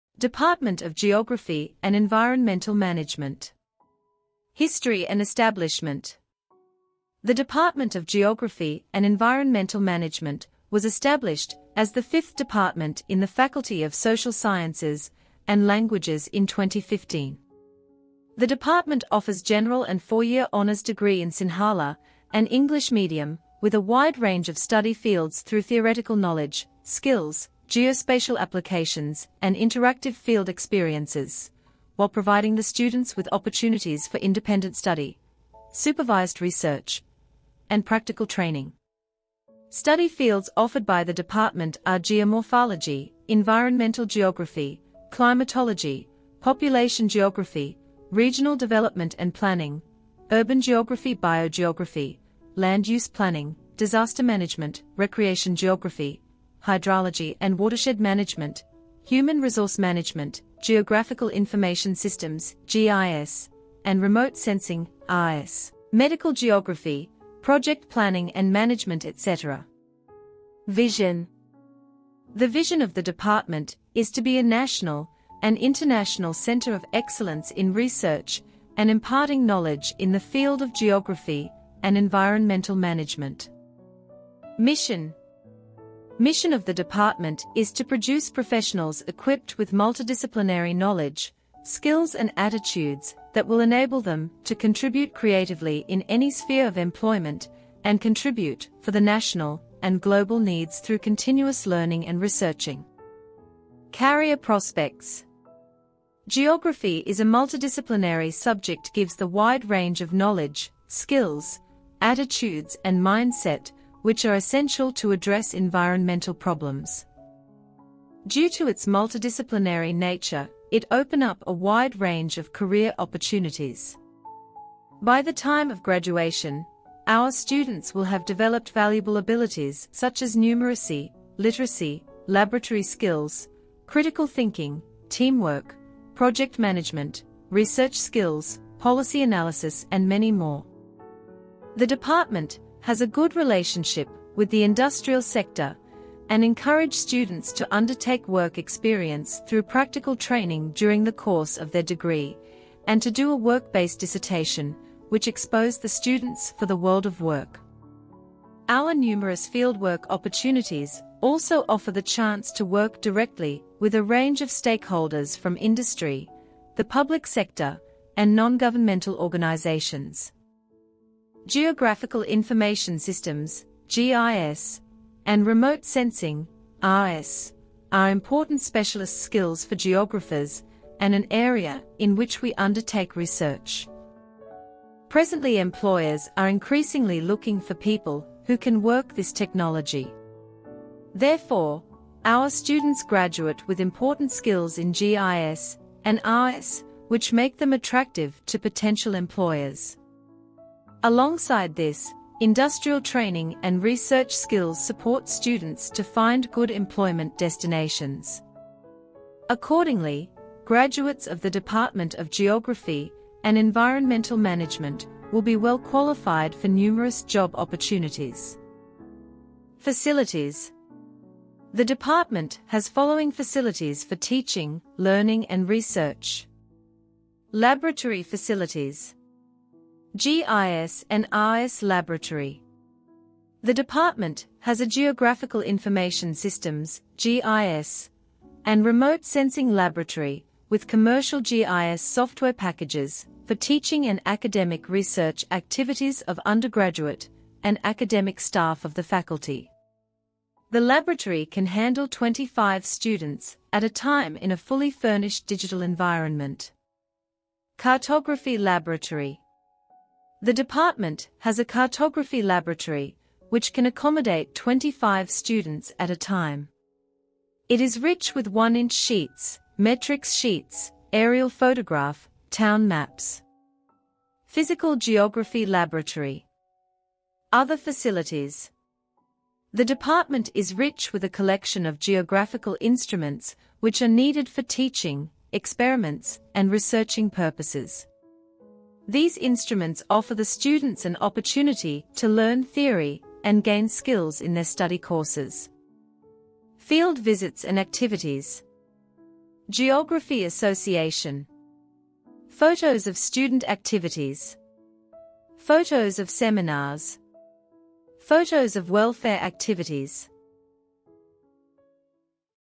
Department of Geography & Environmental Management
The Department of Geography and Environmental Management in the Faculty of Social Sciences and Languages of Sabaragamuwa University of Sri Lanka was established on 18th May 2018. It offers a BA (Hons) in Geography.